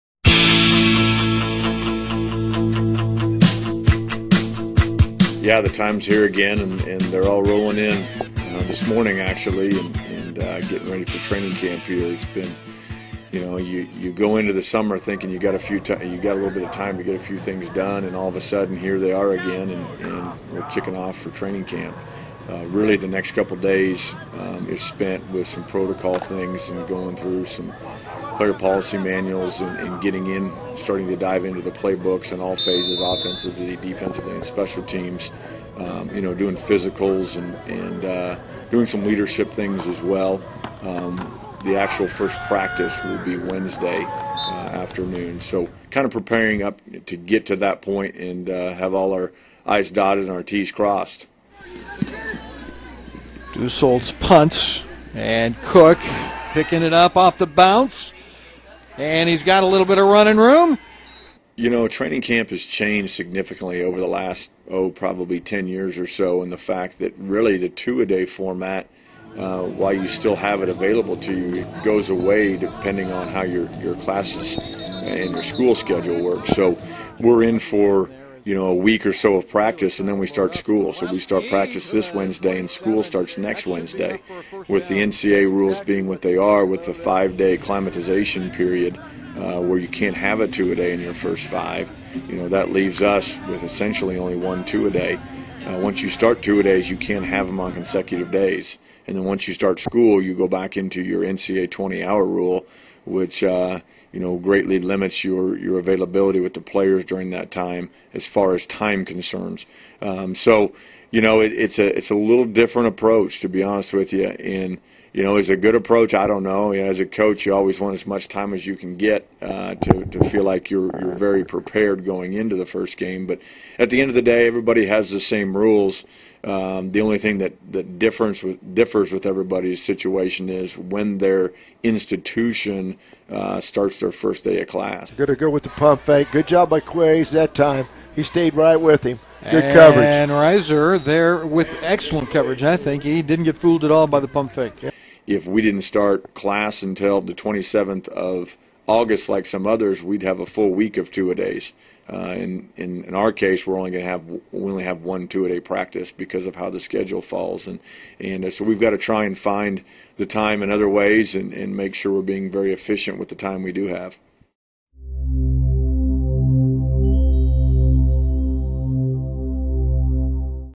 FB Training Camp Opens August 11, 2014
Jerod Kruse, head football coach at William Jewell, talks about the opening of the 2014 fall training camp.